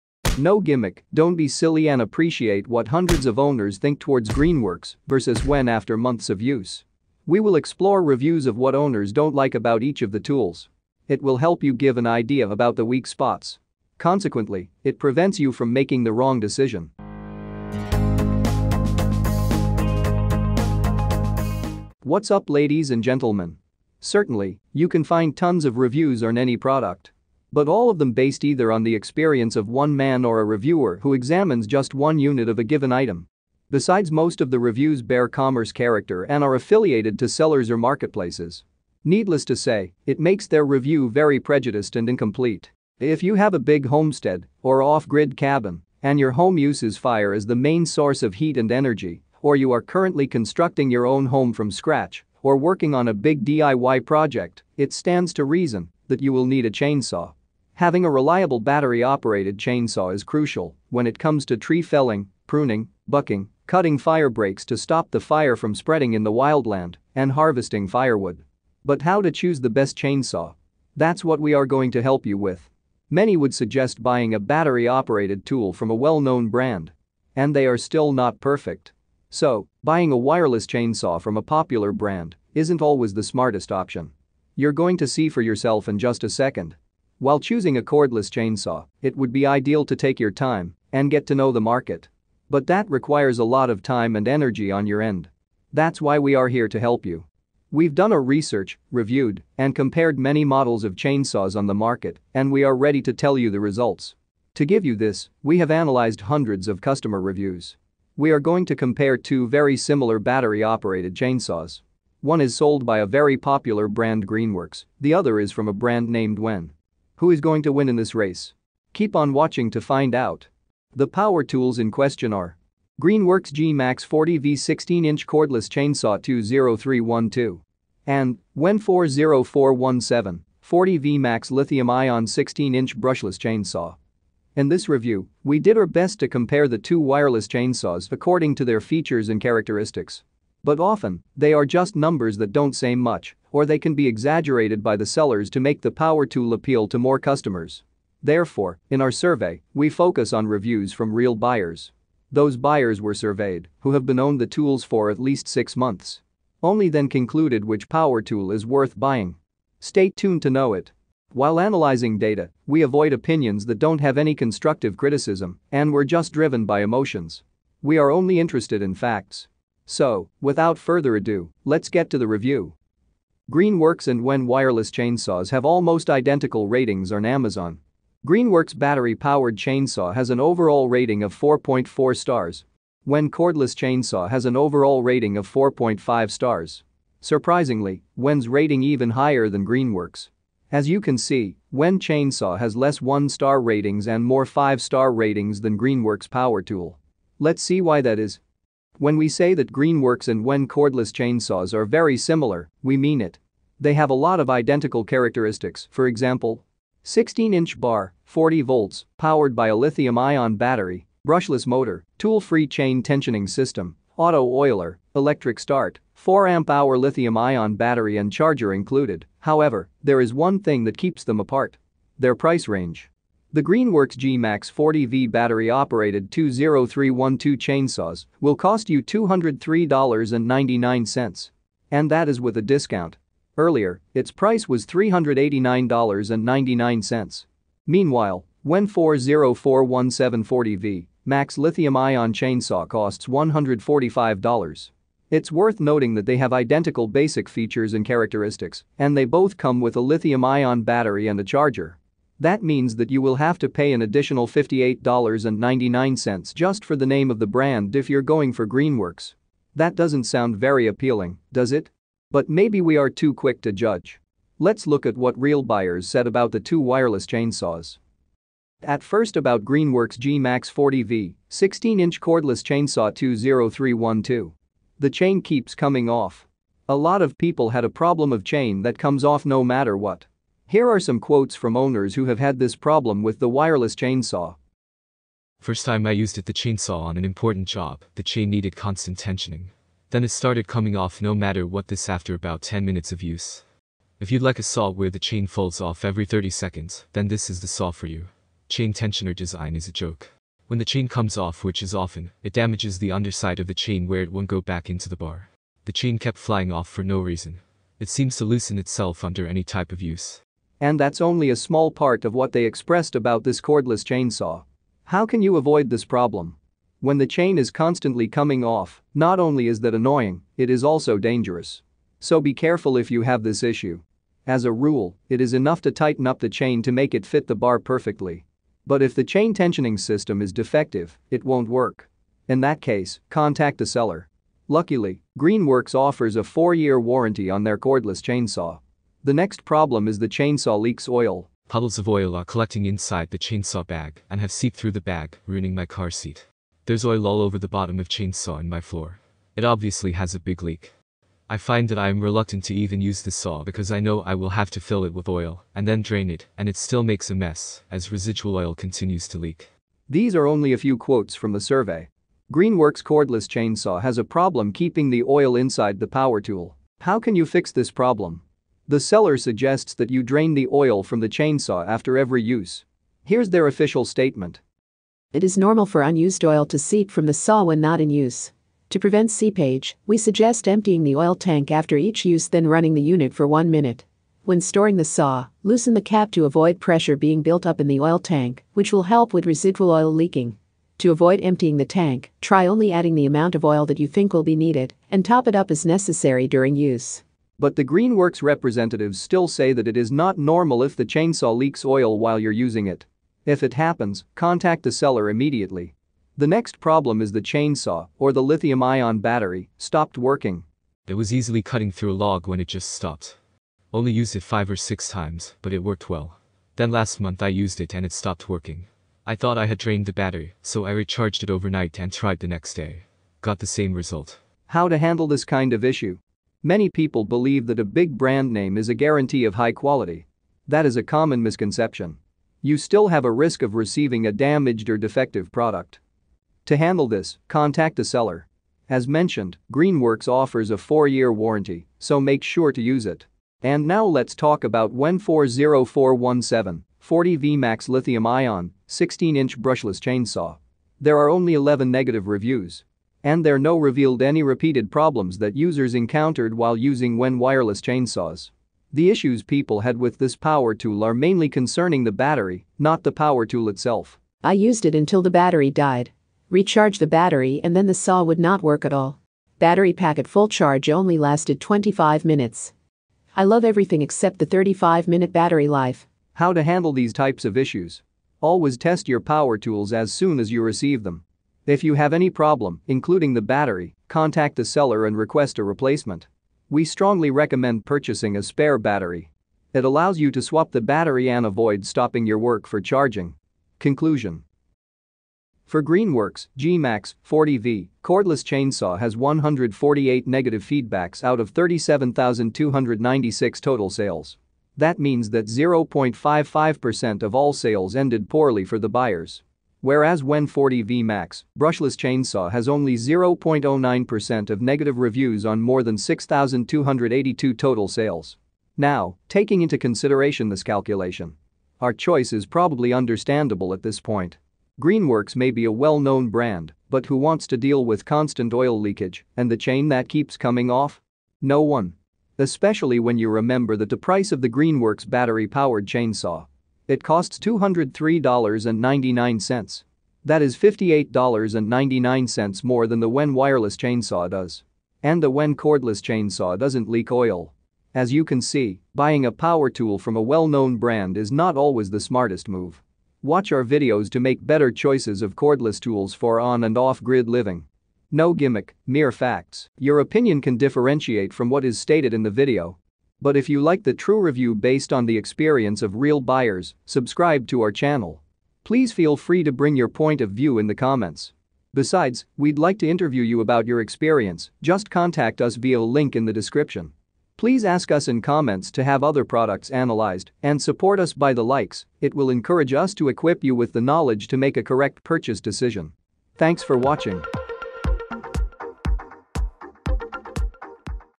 Owners vs affiliates in GreenWorks vs WEN Chainsaw | Survey result vs Jeffostroff & Tool Boss review
What real owners unbiasedly state about GreenWorks Chainsaw. That differentiates from what Tool Boss and The Den of Tools states in their reviews.
Almost everyone living off the grid had a time when it was clear it’s time to buy a chainsaw. And you immediately face another question: which is better — the branded chainsaw or an aftermarket one.
We are going to find out which Chainsaw is better — branded or generic — based on real reviews, as well as learn some life hacks on how to avoid buying low-quality Chainsaw?🛠⚙️🛠

You can find plenty of reviews on chainsaws for off-grid living. Whereas Greenworks chainsaw stands out of other Chainsaw it was not even mentioned in Jeffostroff and Project Farm reviews. How come? Aren’t they good in quality? To find out whether it is true we have examined what the existing owners tell about Greenworks Chainsaw and how it stands vs aftermarket one.
This information allows you to take the right decision.

⏱Timestamps⏱
00:00 Introduction No gimmick, just feedbacks from real people
01:09 How to choose the best chainsaw
01:51 Comparing Chainsaw, Branded and Unknown Brand
02:04 Tools in question
02:54 GreenWorks and WEN Ratings
03:27 GreenWorks and WEN Characteristics
03:43 Prices of Each Tool
04:46 Reviews of Buyers about Greenworks Chainsaw
06:15 Statement of Greenworks Production Team about the Problem
07:06 a few tips on how to avoid problems
07:25 WEN Chainsaw Review
07:44 Problems With WEN CHAINSAW
08:14 Conclusion on GreenWorks and WEN Chainsaw
09:38 The call to share your experience in comments